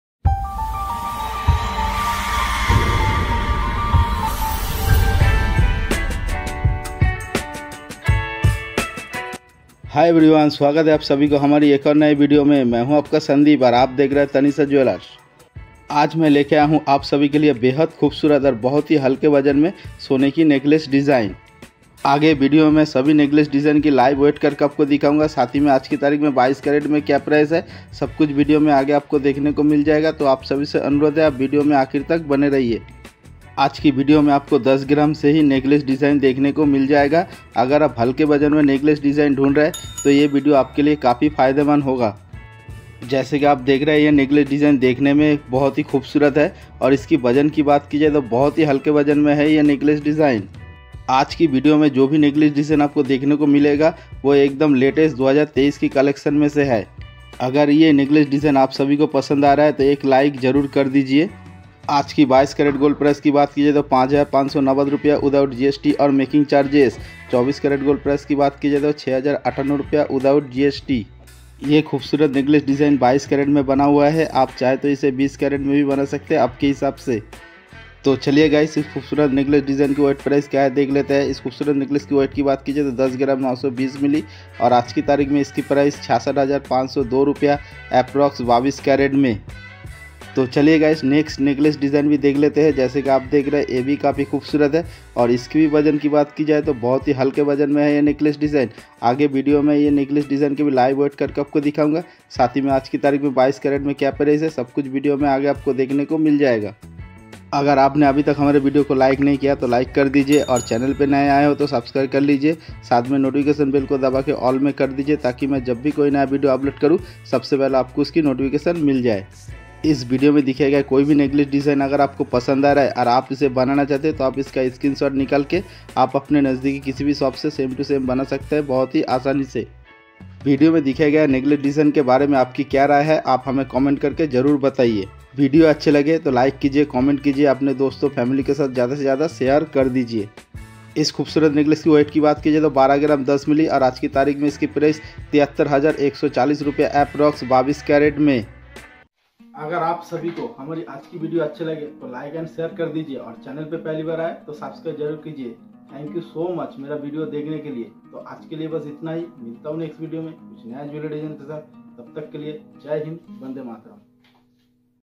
10 Gram Gold Necklace || 10 Gram Gold Necklace Design With Price
10 Gram Gold Necklace || 10 Gram Gold Necklace Design With Price

Hi I am Sandip Ghosh And You Are Watching Tanisha Jewellers
Today I will Show You Some 10 Gram Gold Necklace || 10 Gram Gold Necklace Design With Price

If You Want To Know Anything Jewellery Please Comment.

Sandip Ghosh
Form:- Anjangaon Maharashtra ( India)